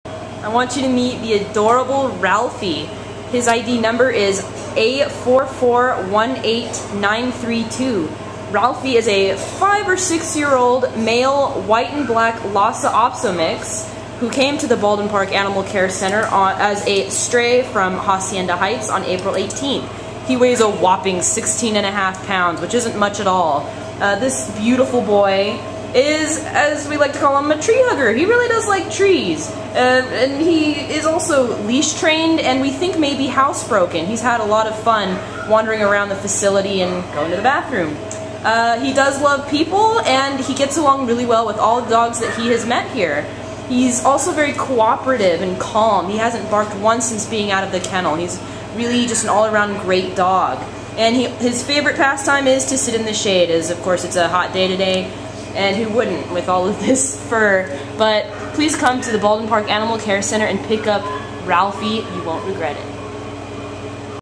Ralphie A4418932 ADOPTED!!!
Ralphie is a calm and cooperative five year old white and black male Lhasa Apso mix who was found in Hacienda Heights on April 18th and brought the Baldwin Park Animal Care Center.  Weighing sixteen pounds, Ralphie walks beautifully on leash, is housebroken and appears to be well-trained in general.  He loves other dogs, and we think he'd be great with kids because he is so unflappable.  Ralphie will be the perfect indoor pet for anybody in any living situation whatsoever; he loves to sit peacefully in the shade and would be a great companion for a retired couple or a senior citizen.